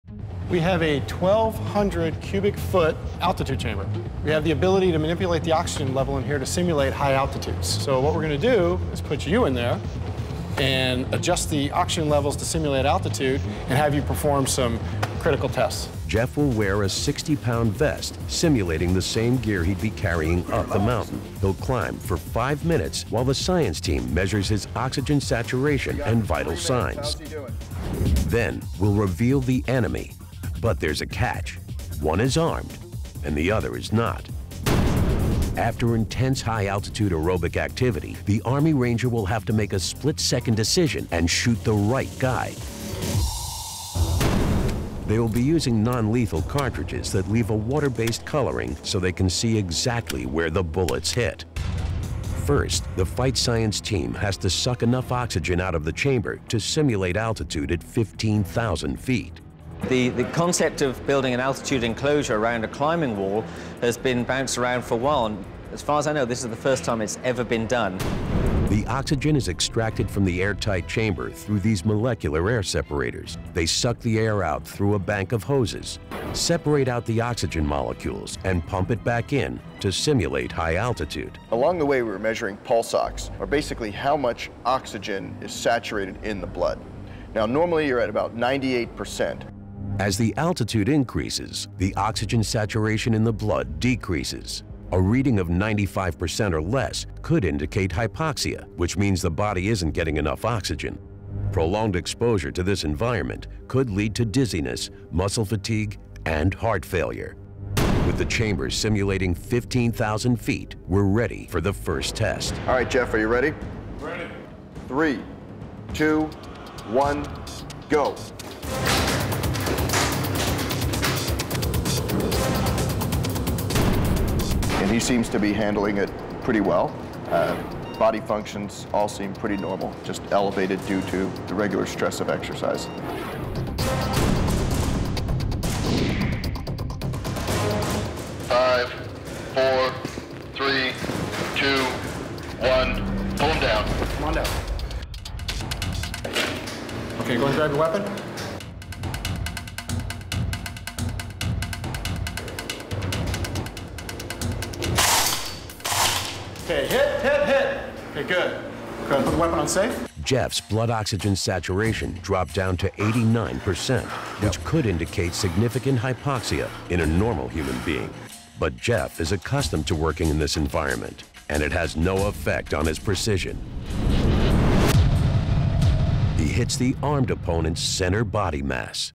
High Altitude Sharp Shooter | National Geographic
This army ranger's elite training helps him shoot on target even when his body is depleted of oxygen.